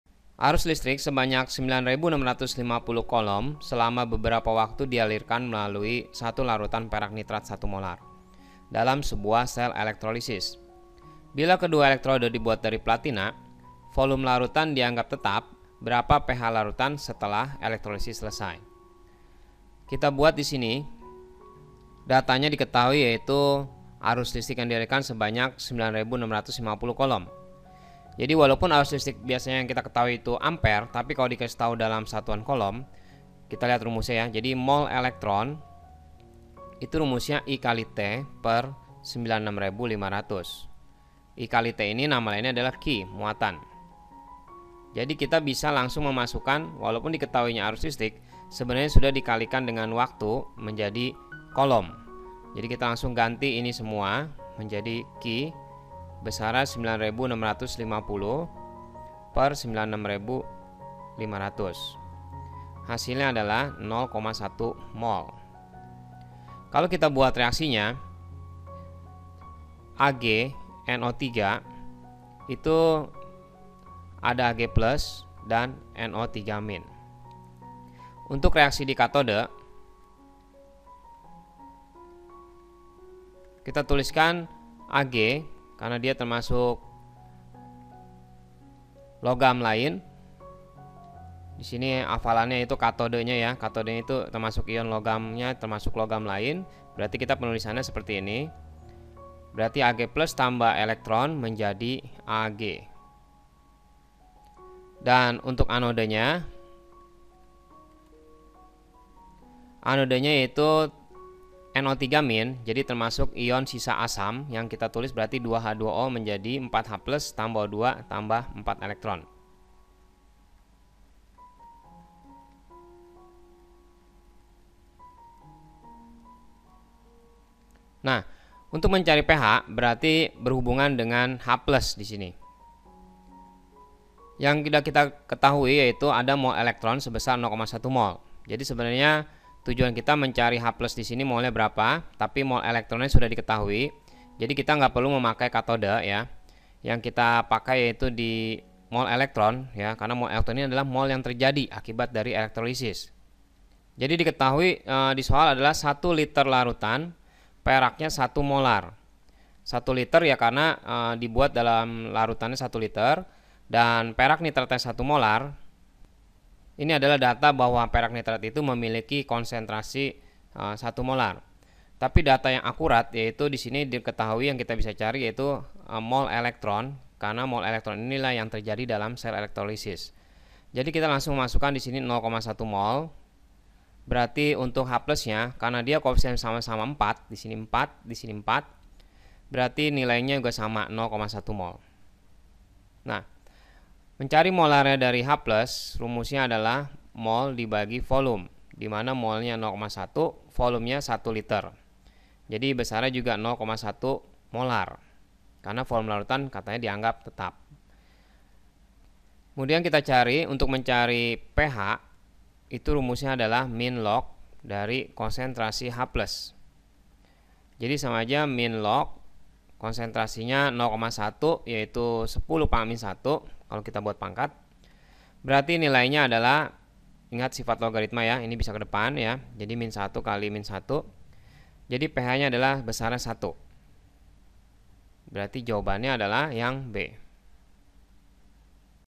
🔴[REDOKS]🔴||Arus listrik sebanyak 9,650c selama beberapa waktu di alirkan melalui 1liter larutan A
Arus listrik sebanyak 9,650c selama beberapa waktu di alirkan melalui 1liter larutan AgNo3 dalam sebuah sel elektrolisis. jika kedua elektroda di buat dari platina dan volume larutan di anggap tetap, berapa ph larutan setelah elektrolisis selesai#elektrolisis#redoks